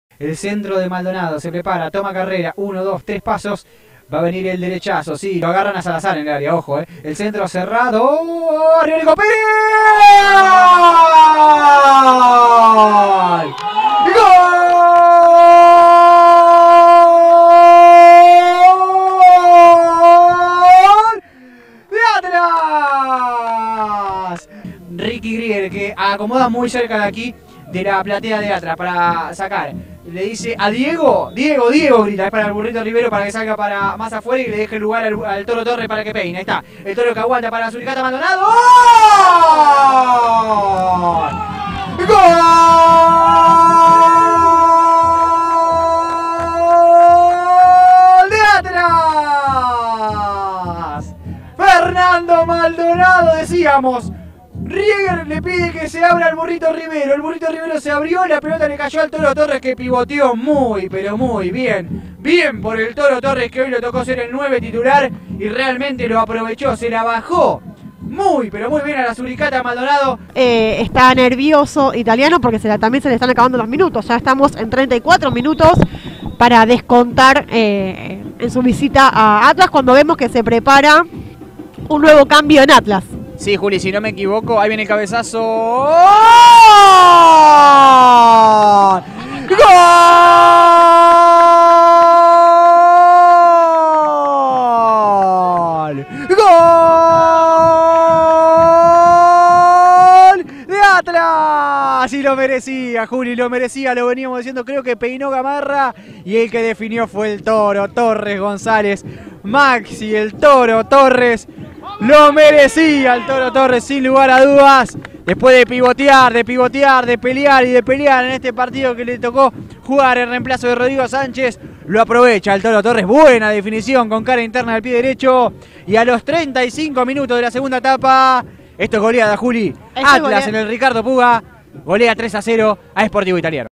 SHOW DE GOLES
TRIPLE SABOR🔥
Los goles de un partido que encendió la confianza al máximo en los guerreros de Néstor Retamar ⚽️
El "topo Gigio" Nico Pérez, la "suricata" Fernando Maldonado, y el "toro" Maxi Torres, para poner al marrón en zona de Reducido💪
Impecable transmisión de @VeoMasOk ⭐️

Gracias por elegir nuestro contenido!